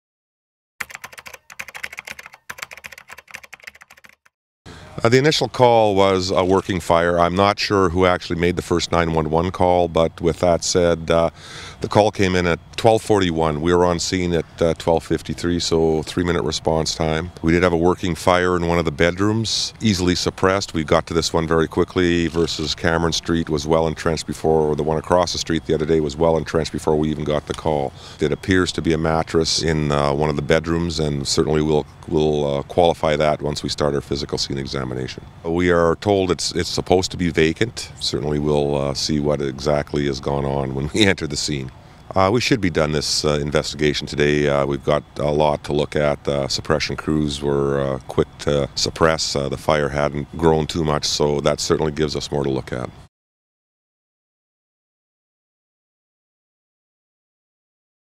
Crews respond to second fire on Cameron Street this week
Crews responded to a fire in the bedroom of an unoccupied home in the 1400 block of Cameron Street on Wednesday. The fire happened just across the street from a home that was severely damaged by a fire on Monday.